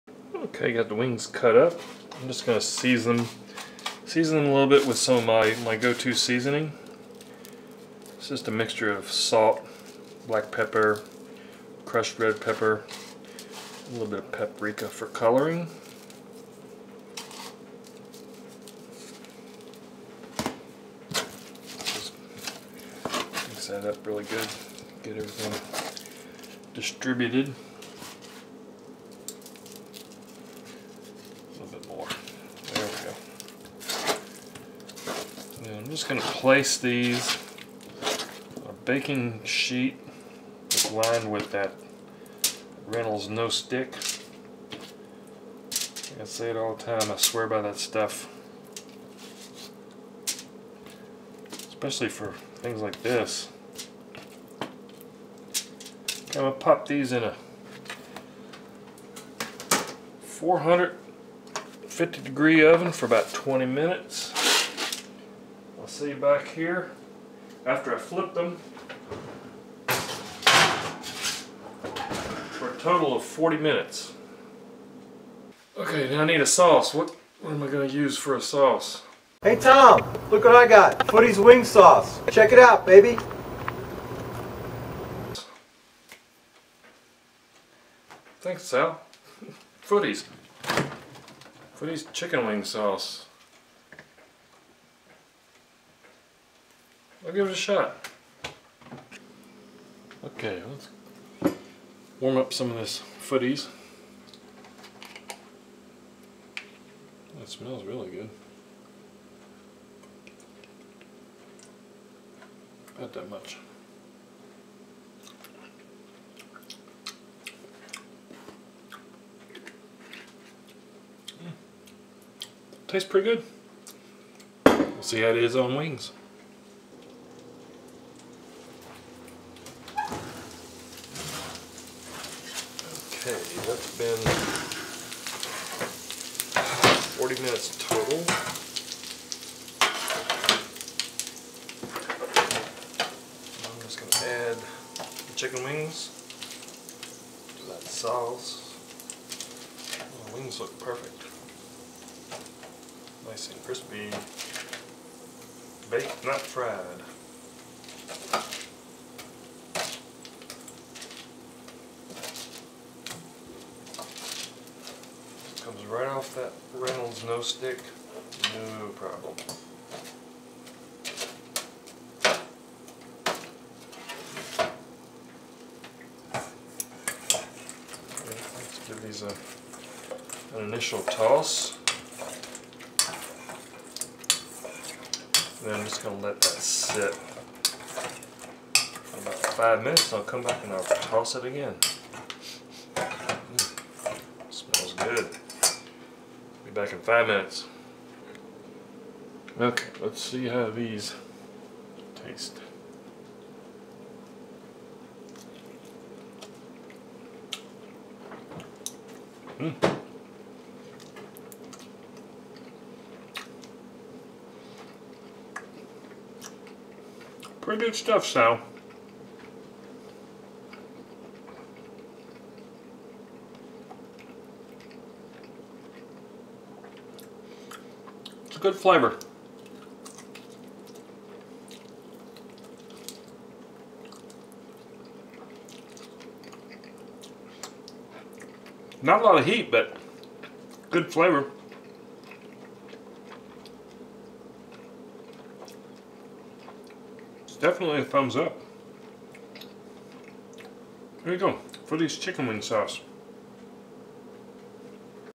Footy's Wing Sauce
Sal from "The Rootboy Cooks" helps me out with some chicken wings.
He has some awesome recipes and beer reviews inkidreferralcode tomstestkitchen